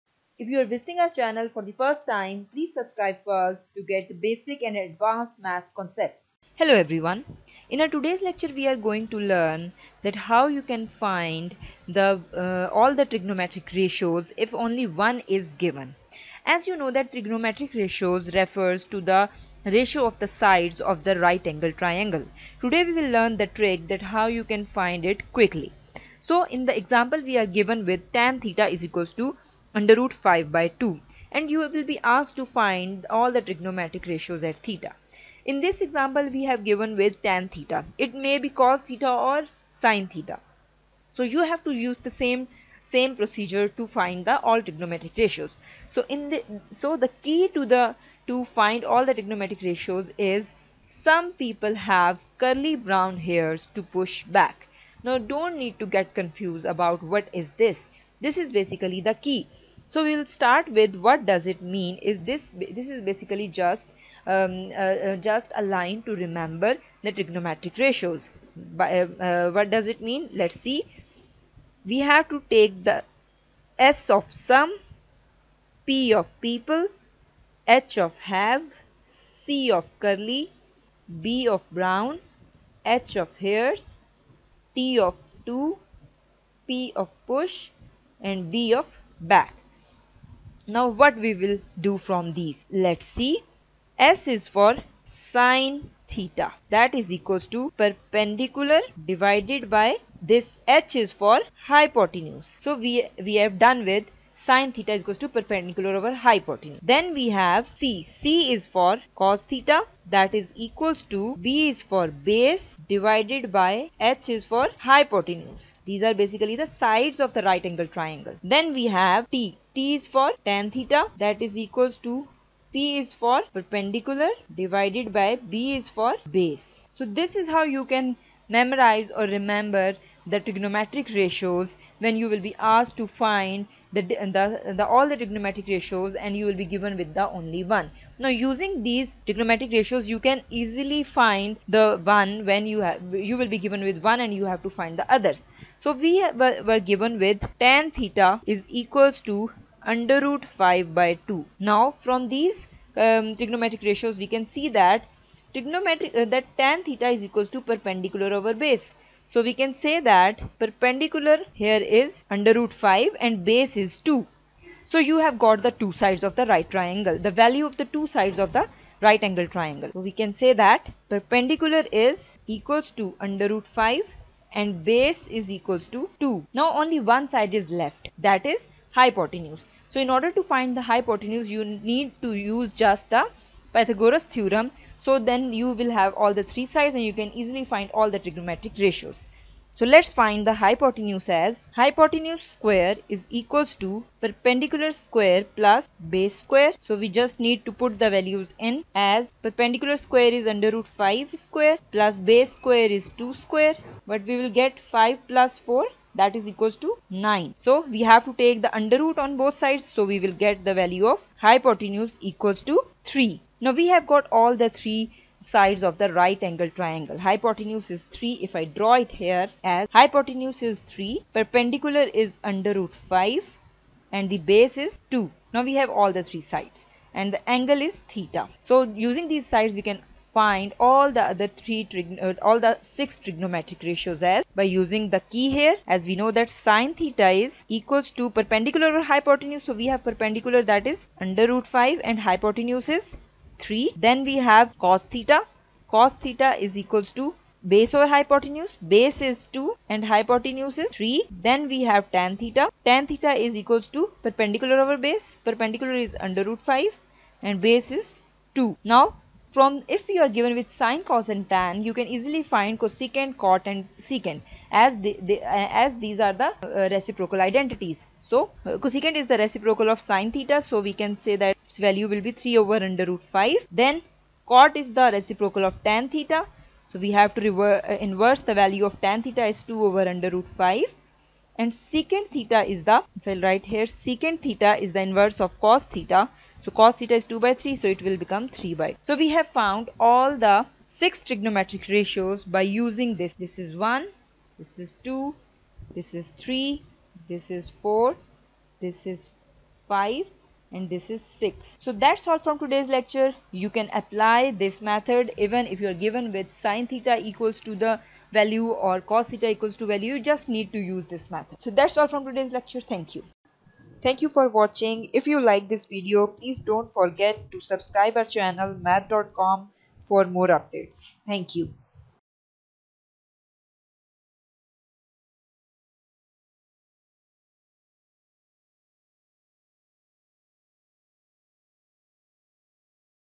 How to Find Value of All Trigonometric Ratios if One is Given | Math Dot Com
How to find all trigonometric ratios if any one is given is explained with example.
Trigonometric ratios are basically the ratio of the sides of right angled triangle.If any one trigonometric ratio is given with two sides of right angled triangle than the third side can be found by using pythagoras theorem.Once you have all the three sides of right angle triangle ,you can easily find all the trigonometric ratios using hypotenuse ,base and perpendicular values.